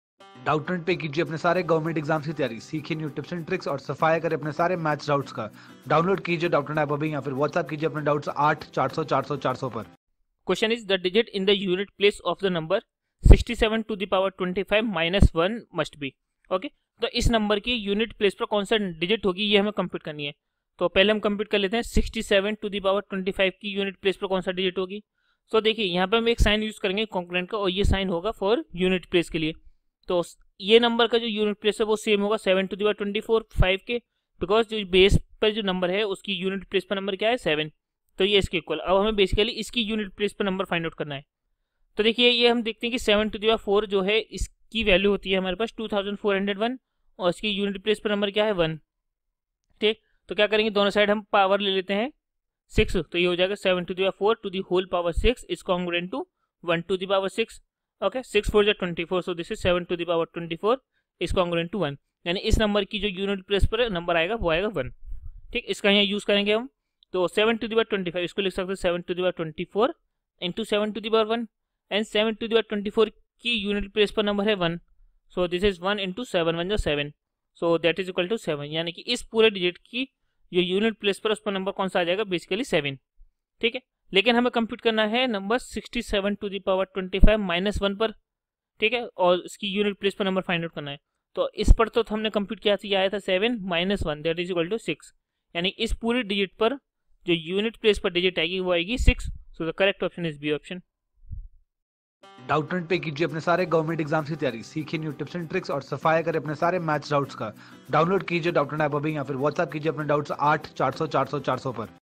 The digit in the units place of the number `(67)^(25)-1` must be 0 (b) 6 (c) 8 (d) None of these
Question From -  Quantitative Aptitude By RS Aggarwal Chapter 2 NUMBER SYSTEM  for  Bank PO,SBI PO, IBPS PO,SSC, Railway Entrance & RBI Exam.